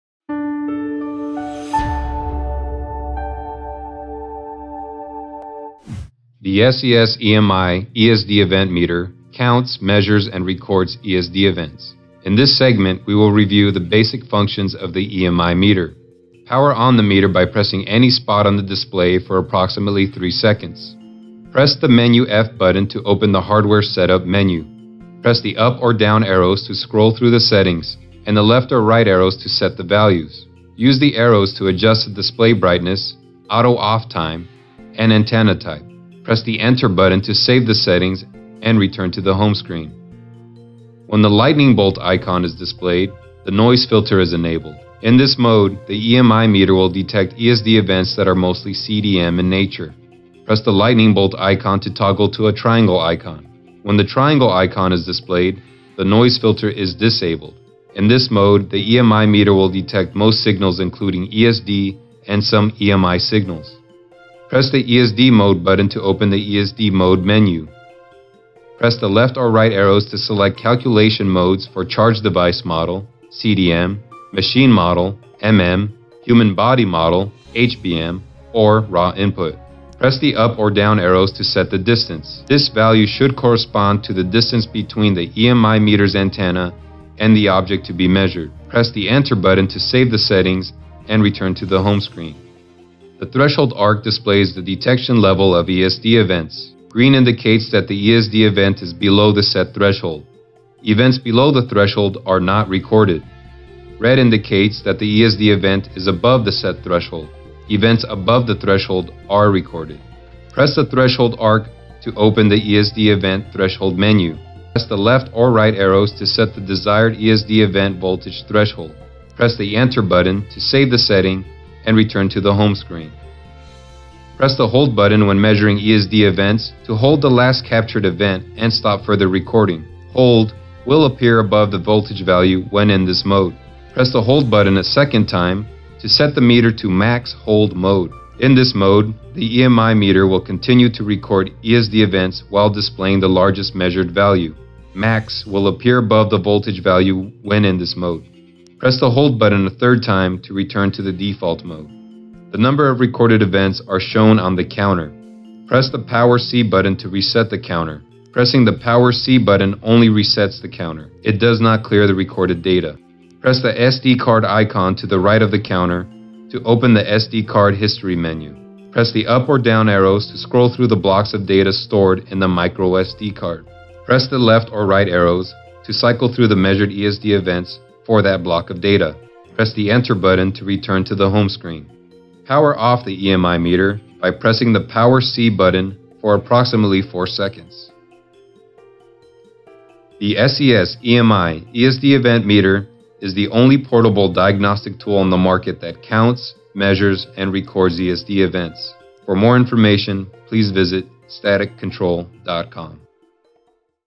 SCS EM Eye - ESD Event Meter Product Walk-through
An introduction to the basic features and functions of the SCS EM Eye - ESD Event Meter